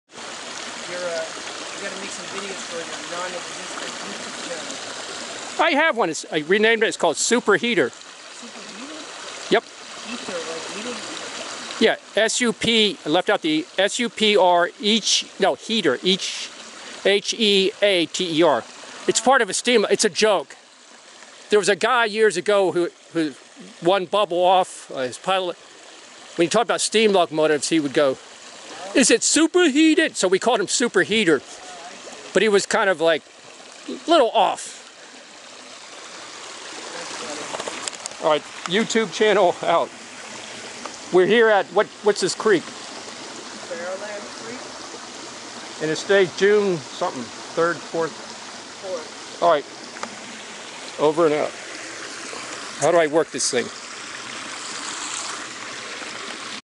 Ricardo & me pausing by the creek during Fairland mountain biking
Short break discussing my YouTube new name during mountain biking at Fairland Regional Park in Montgomery County, MD